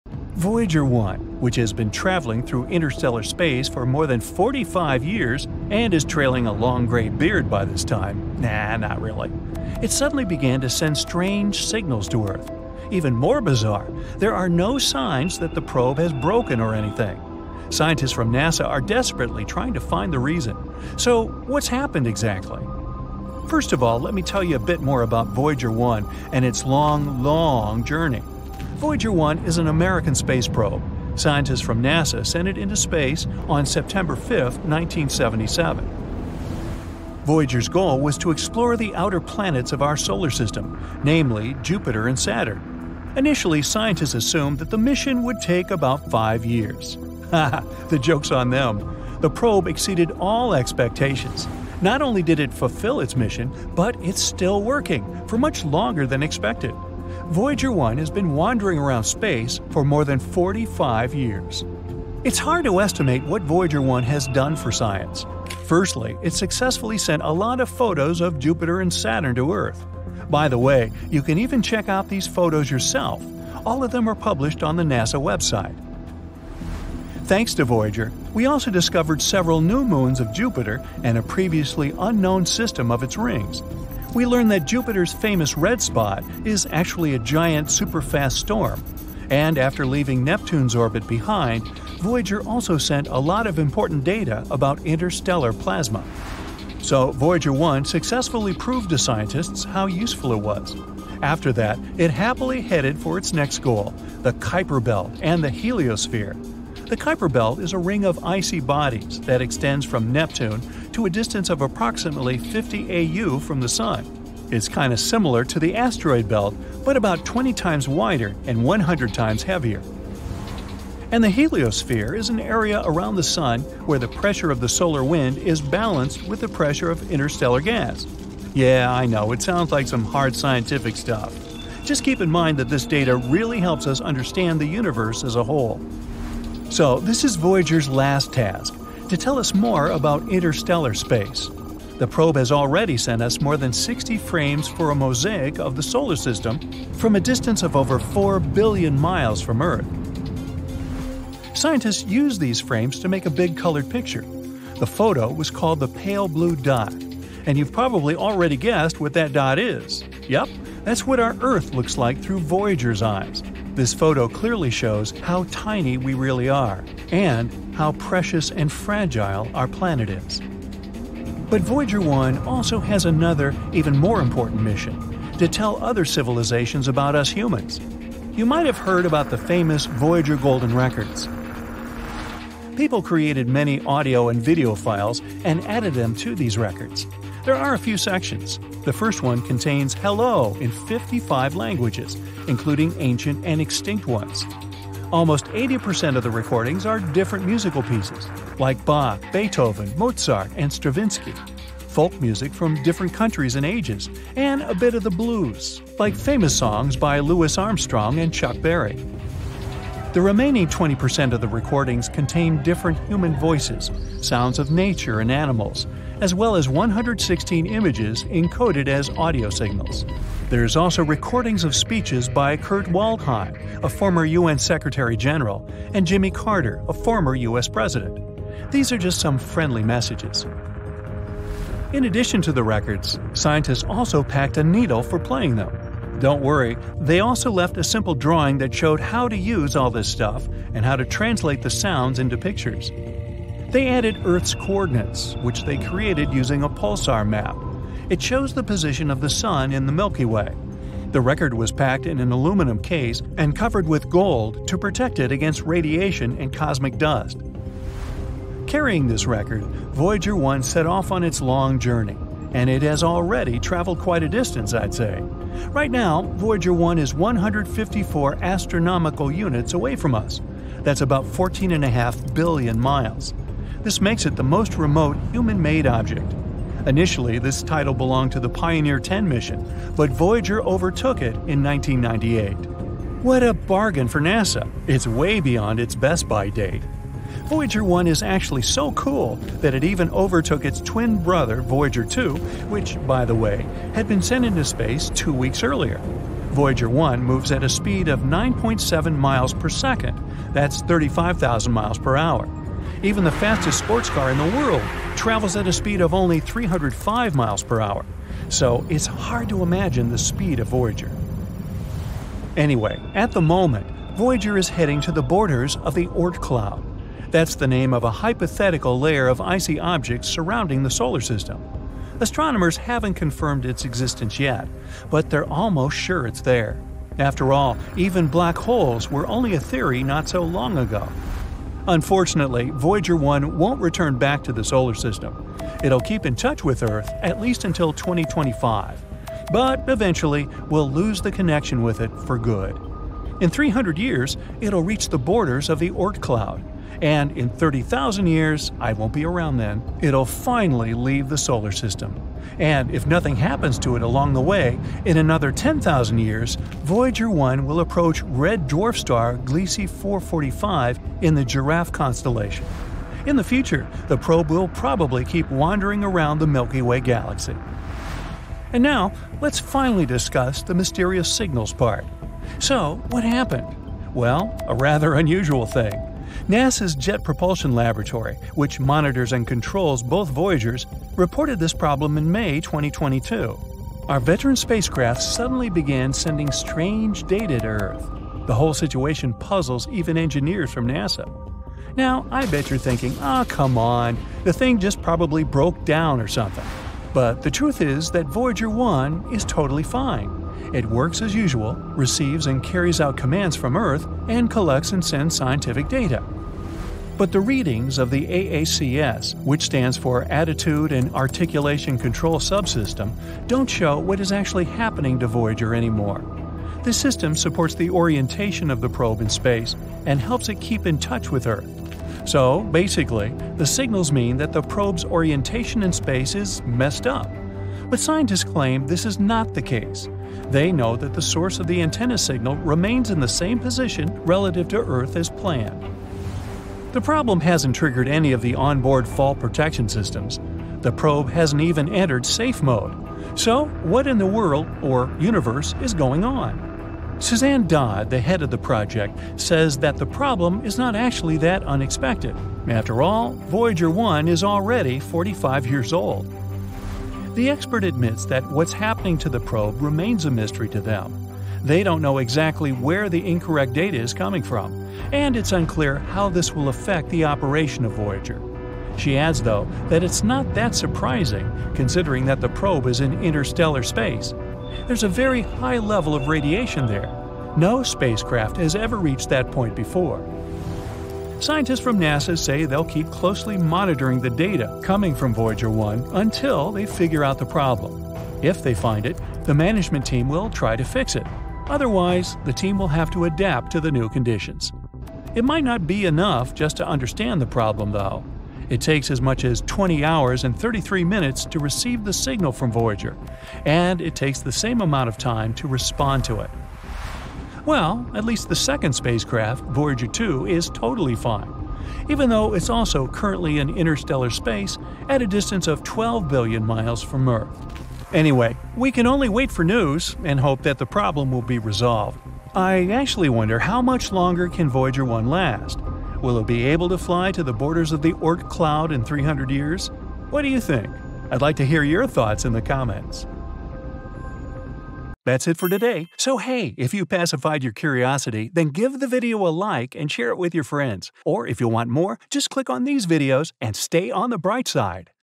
Bizarre Signals from Voyager 1: What Are They?
Voyager 1, which has been traveling through interstellar space for more than 45 years, suddenly began to send strange signals to Earth. Even more bizarre, there are no signs that the probe has broken or anything. Scientists from NASA are desperately trying to find the reason. Thanks to Voyager, we discovered several new moons of Jupiter and a previously unknown system of its rings and learned that Jupiter's famous Red Spot is actually a giant, super-fast storm. So, what could be producing these mysterious signals?

Credit:
NASA/Matt Carasella Photography
NASA/JPL-Caltech